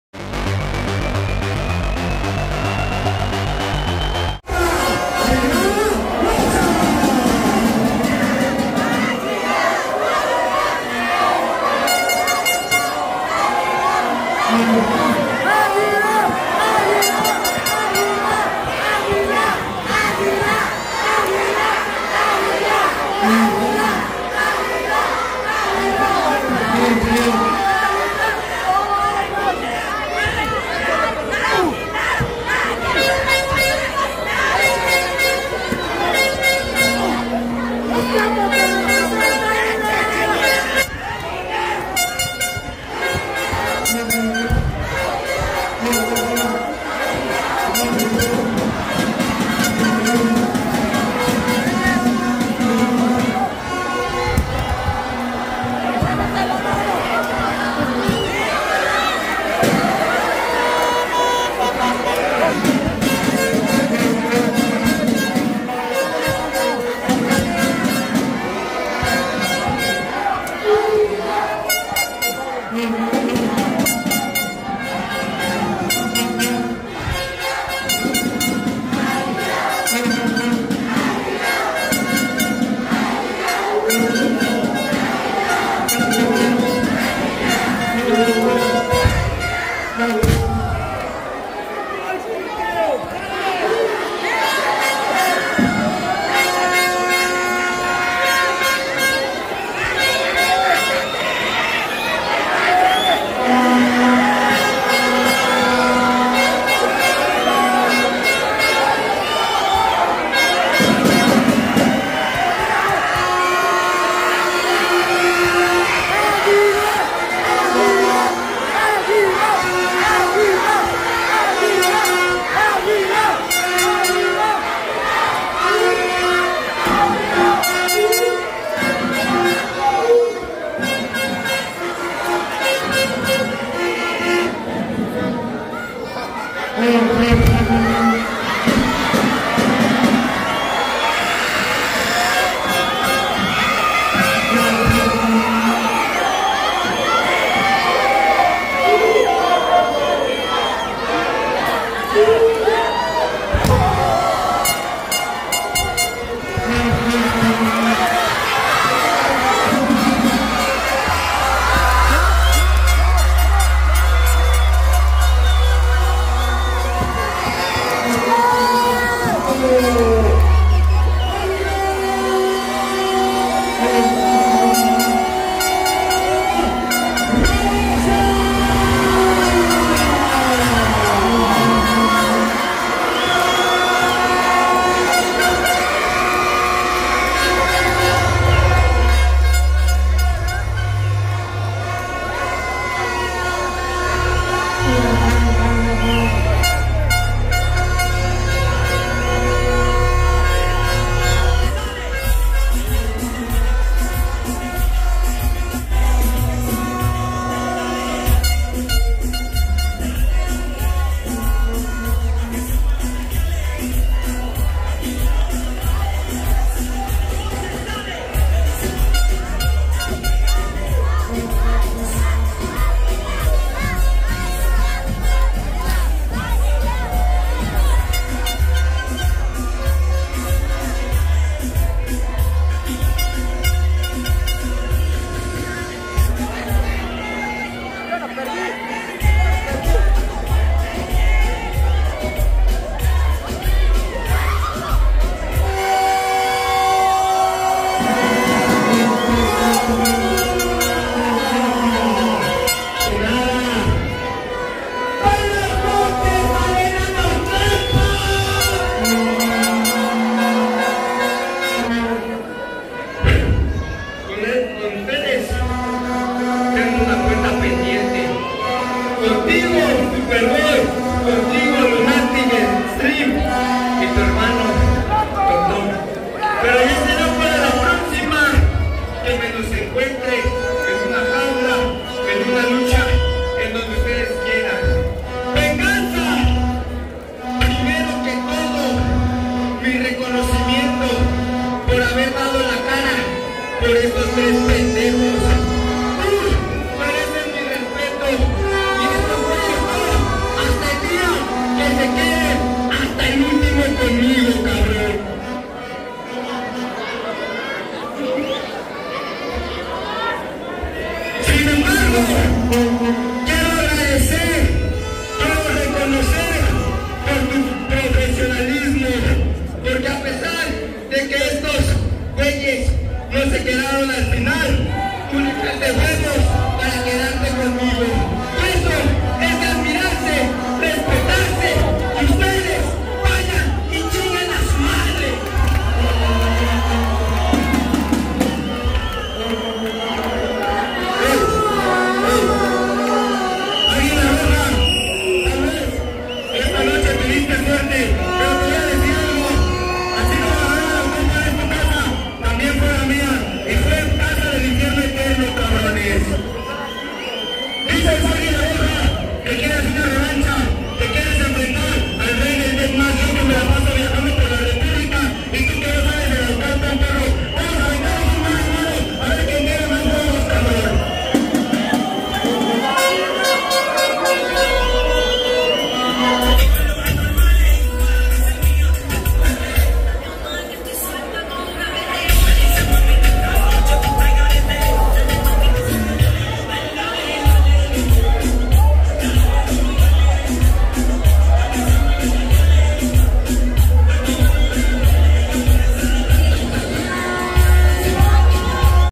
CABELLERA VS CABELLERA: ÁGUILA ROJA VS VENGANZA
JUEVES 16 DE MAYO DEL 2024. ARENA NAUCALPAN IWRG  Y LUCHA LIBRE BOOM